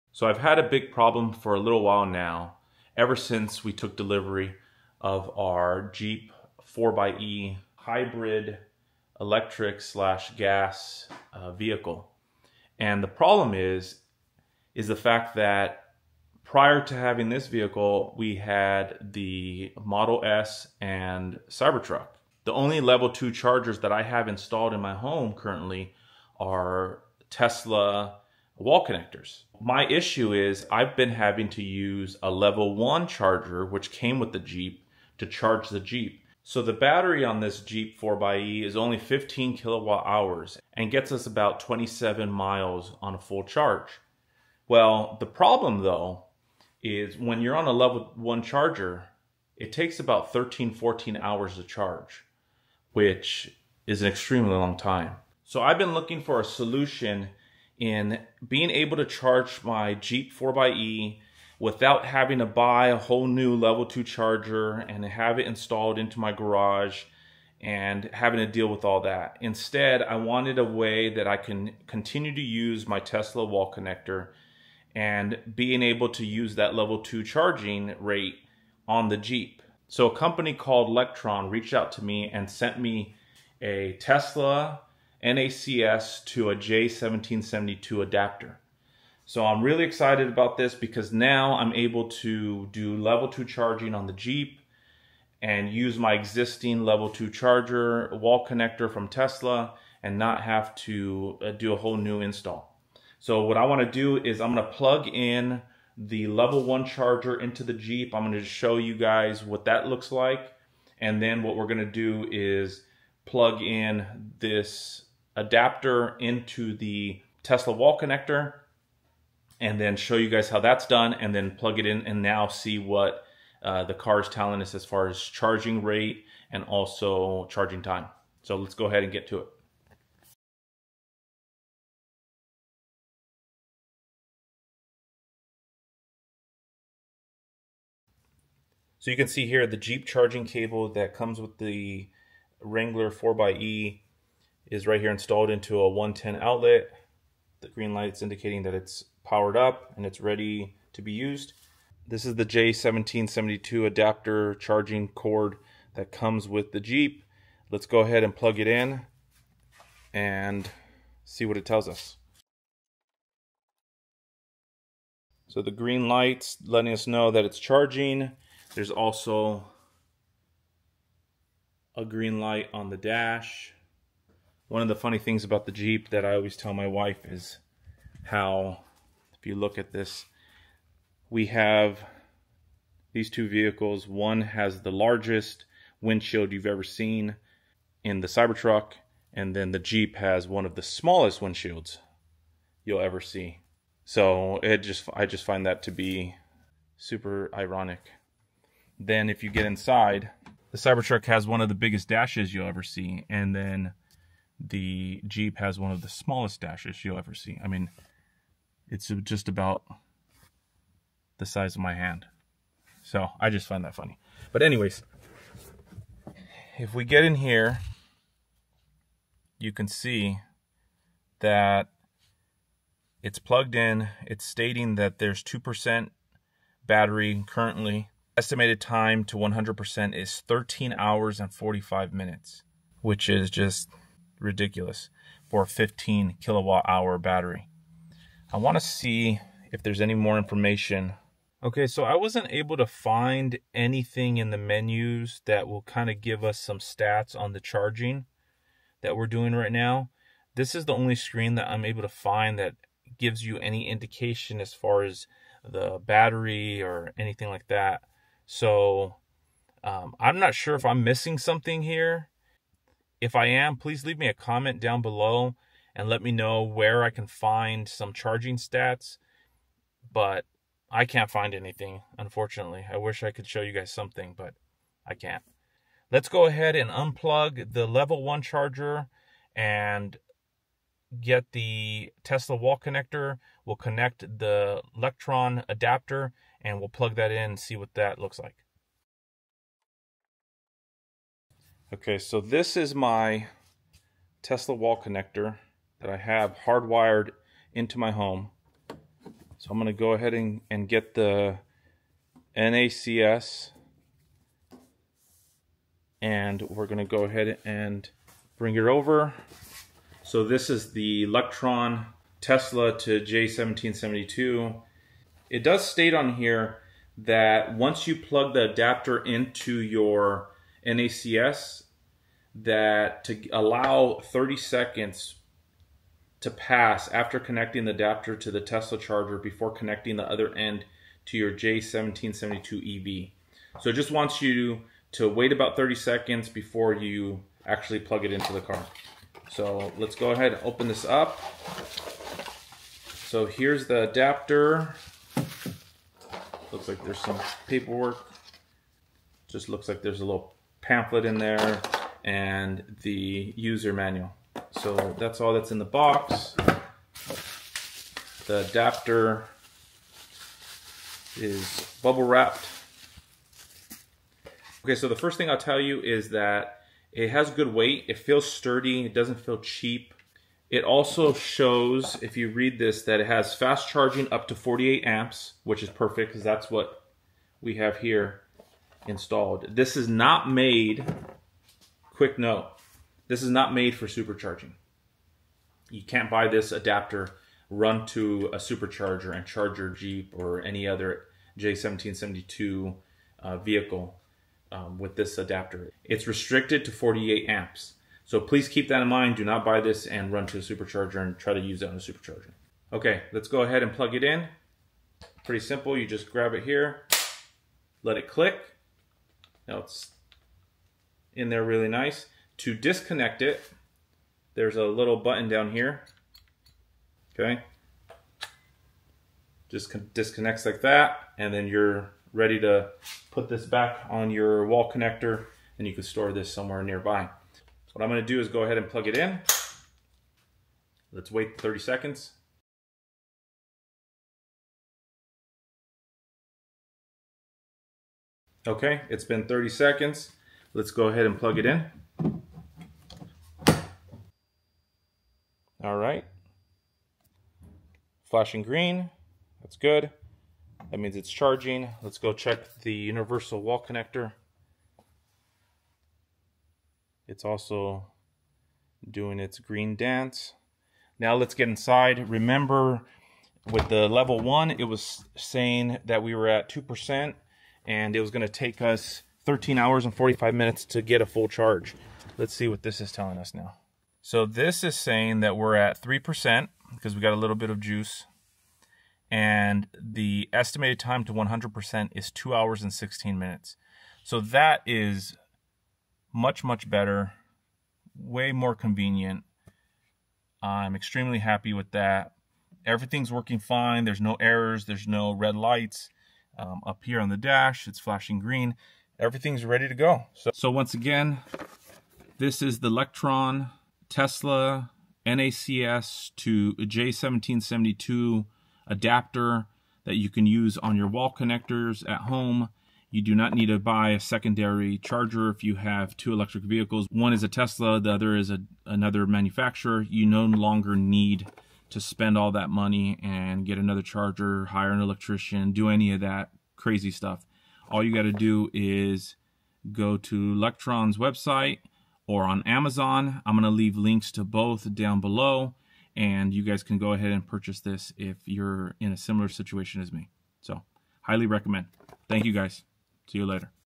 Charge any EV with your Tesla Wall Connector
Tesla Wall Connector

In today's video, we demonstrate how to charge your non-Tesla electric vehicle using your home Tesla Wall Connector with the Lectron NACS to J1772 Adapter. If you own an electric vehicle that isn't a Tesla and have a Tesla Wall Connector installed at home, this video is for you!

🔍 What You'll Learn:
Introduction to the Lectron NACS to J1772 Adapter: Discover the key features and benefits of this versatile adapter.
Compatibility: Learn which EV models can be charged using this adapter.
Step-by-Step Guide: Follow our easy-to-understand instructions to connect and charge your EV seamlessly.
Real-World Testing: Watch as we test the adapter with different non-Tesla EVs to ensure optimal performance.
Pros and Cons: Get our honest review of the adapter, including its strengths and any limitations you should be aware of.